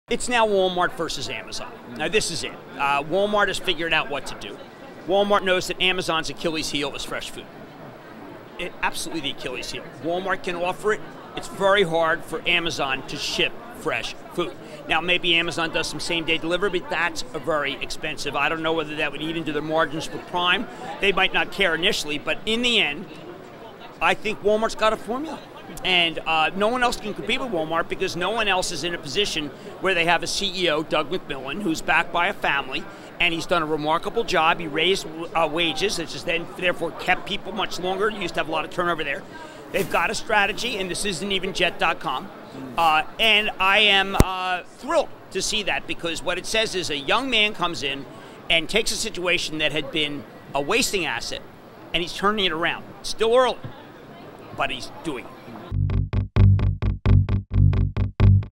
Jim Cramer: Walmart Knows Amazon's Achilles' Heel Is Fresh Food
TheStreet's Action Alerts PLUS Portfolio Manager Jim Cramer said Walmart knows that Amazon's Achilles' heel is fresh food.

"It's now Walmart vs. Amazon," Cramer said. "Walmart has figured out what to do."